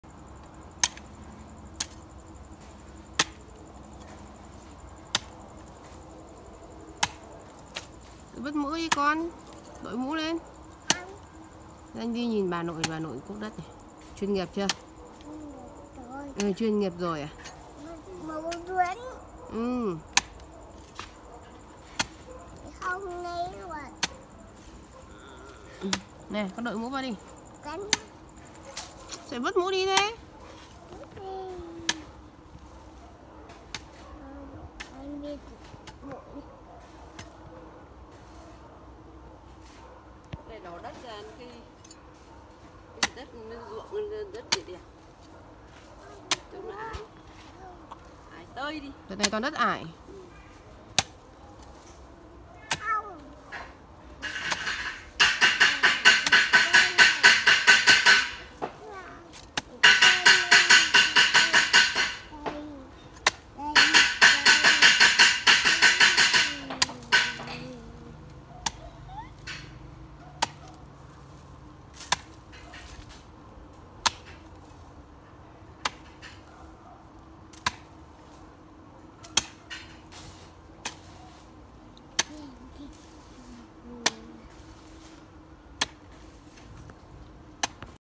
Cách cuốc đất vườn, đất ruộng để trồng Ngô Nữ Hoàng
Đây là clip hướng dẫn mọi người cách cuốc đất ở ruộng, vườn để trồng Ngô hiệu quả nhất